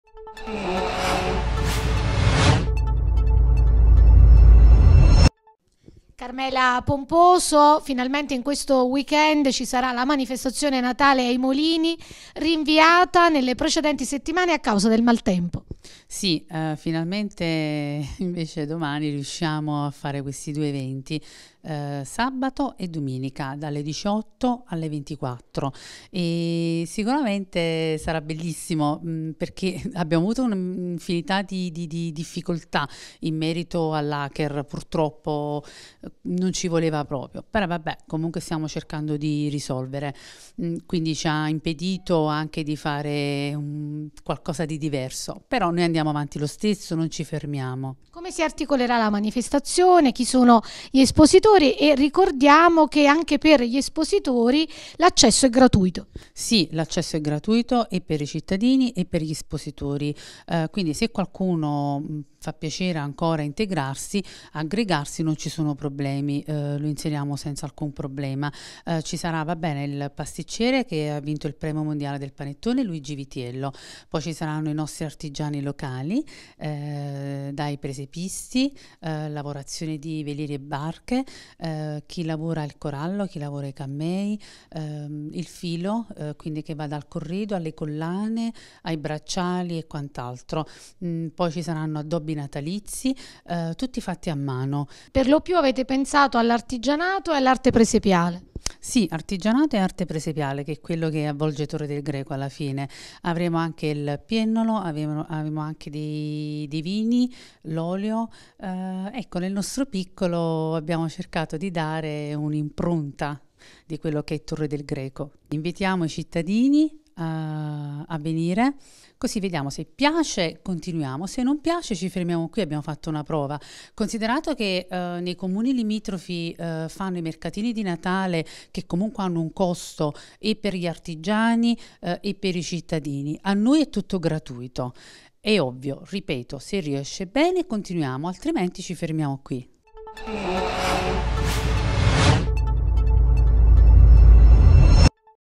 Natale ai Molini, weekend di eventi nell'area di Via Calastro
Sabato 3 dicembre e domenica 4 dalle 18:30 nell'area di Via Calastro ci sarà "Natale ai Molini".

Esposizione a cura di artigiani e maestri presepiali, musica e degustazioni di prodotti agricoli e pescato locale.

Ai microfoni di TVCITY Carmela Pomposo, consigliera al benessere cittadino, presenta l'evento.